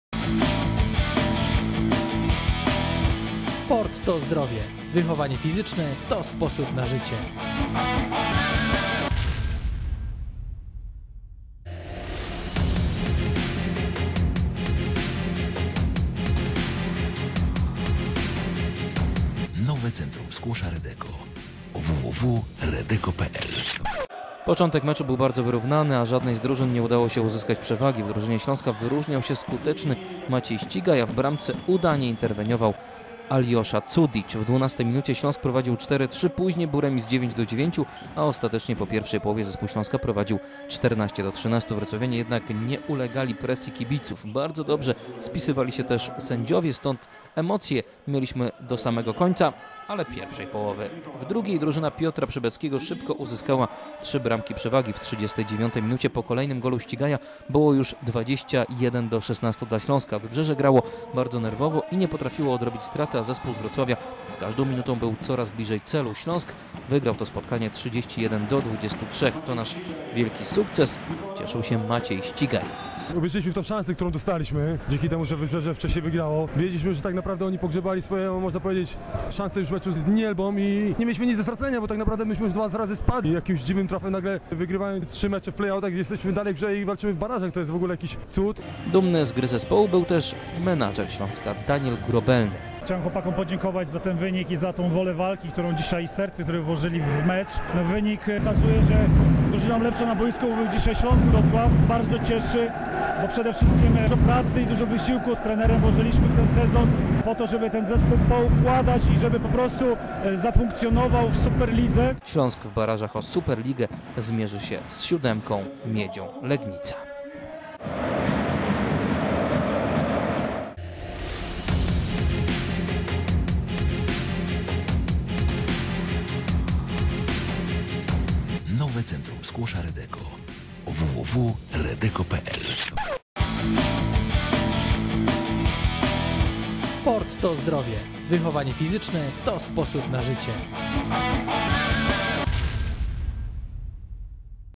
Piłkarze ręczni Śląska zagrają w barażach o Superligę!
Dwa razy byli już spadkowiczami. W decydujących momentach jednak pomógł los, a i swoje zrobili na parkiecie. Piłkarze ręczni Śląska Wrocław pozostają wciąż w grze o superligowy byt. W meczu ostatniej szansy drużyna Piotra Przybeckiego pokonała na wyjeździe Wybrzeże Gdańsk 31:23.